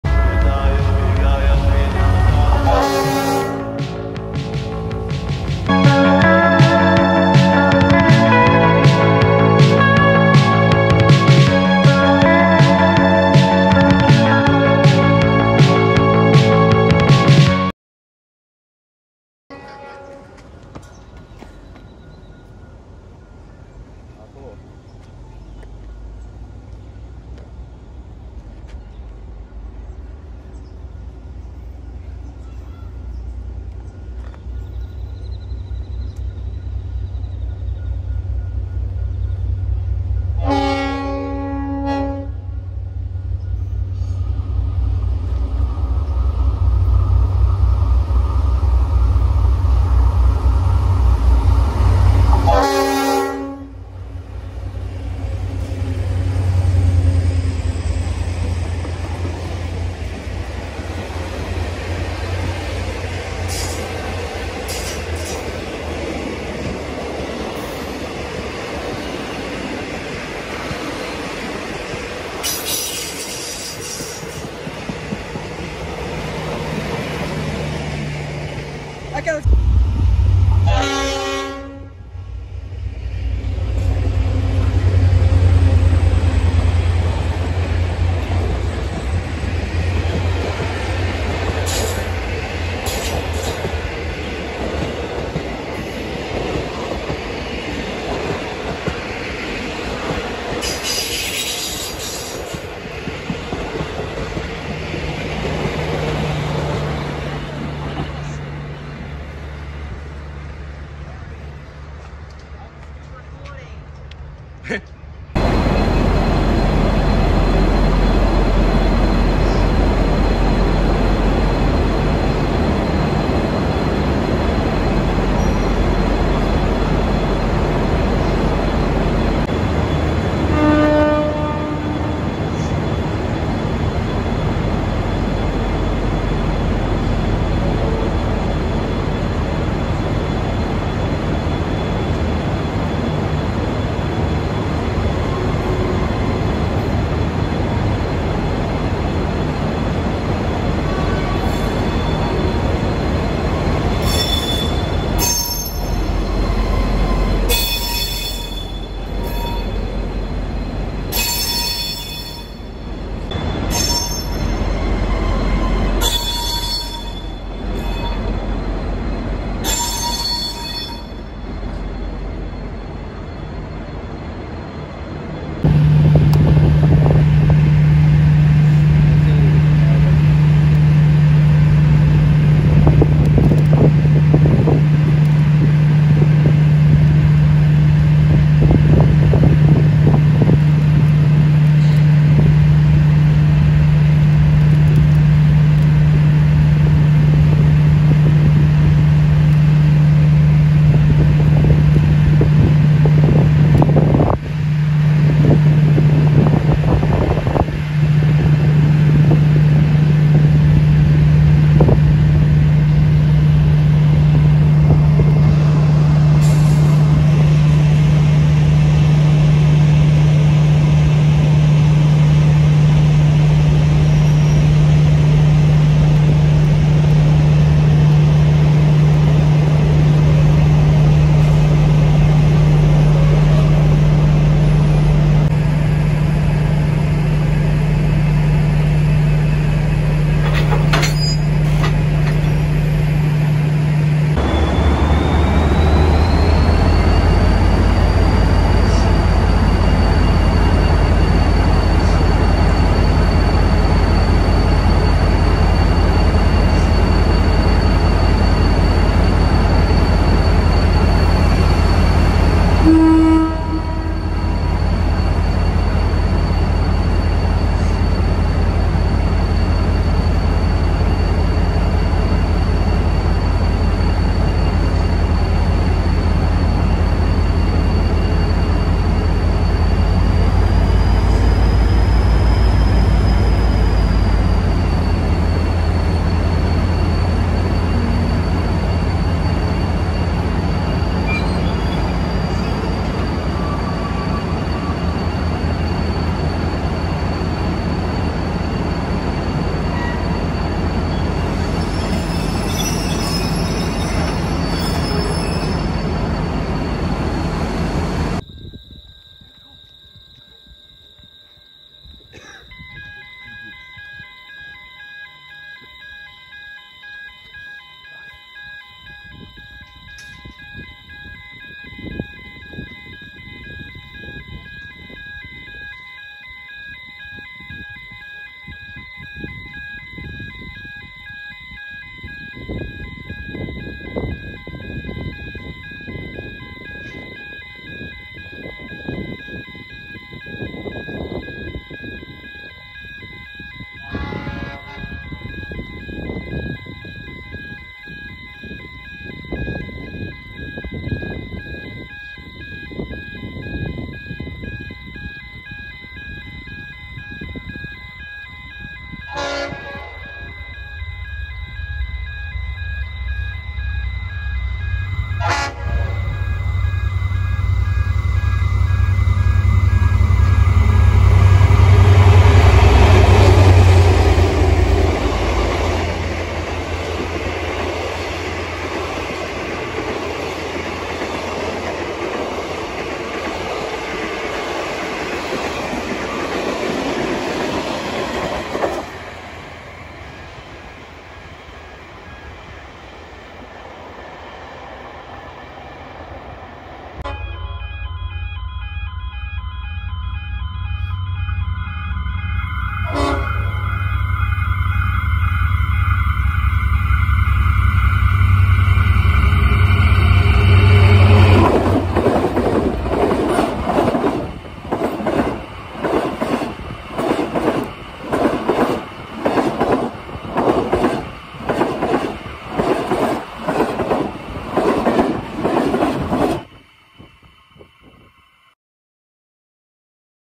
A66 N464 hauling the Grainlander
I chase 707 Operations Grainlander tour, their first of the year, seen at Yarraville station, SCS and Little River station hauled by V/line's A66 and N464. First 2 locations seen with @trainspottingforfunwithharry @buzzytrains9037 @DINO223 .

Filmed 17/2/23